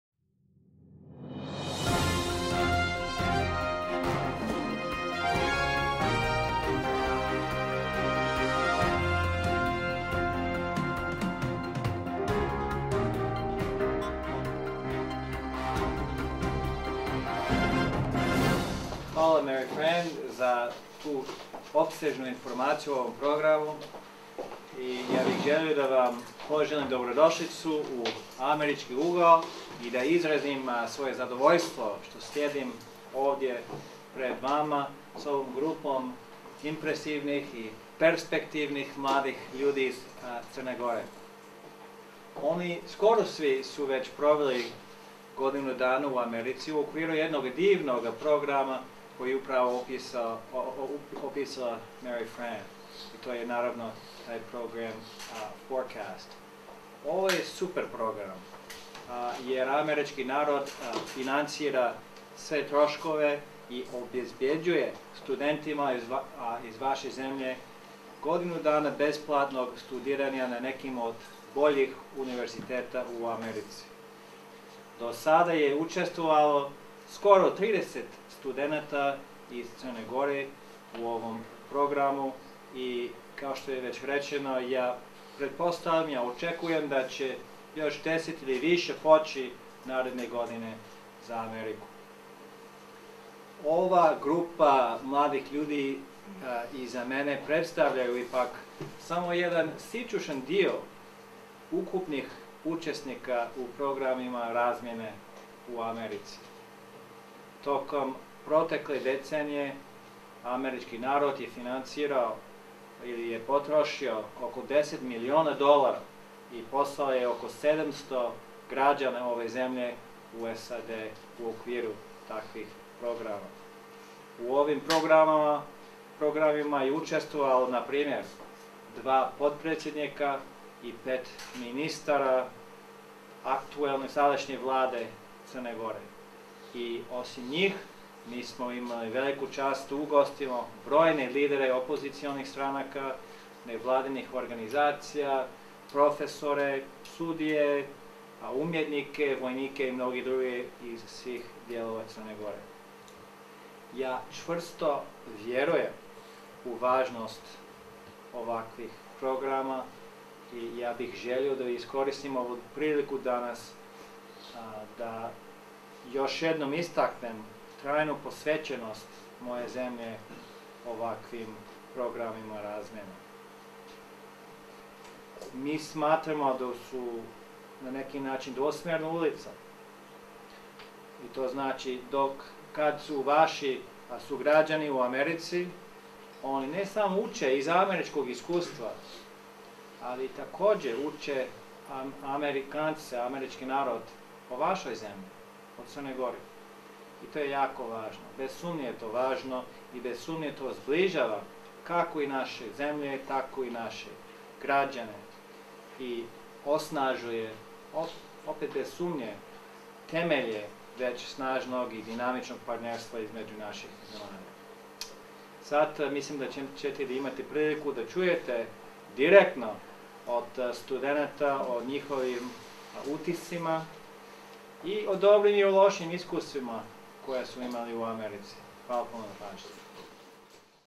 Ambassador Moore Discusses Educational Exchanges
On November 4, U.S. Ambassador to Montenegro Roderick W. Moore discussed the importance of international education and educational exchanges in a press event at the American Corner, KIC "Budo Tomovic" in Podgorica. Ambassador Moore presented some of the Montenegrin students who studied at U.S. universities in the last two years through the FORECAST program.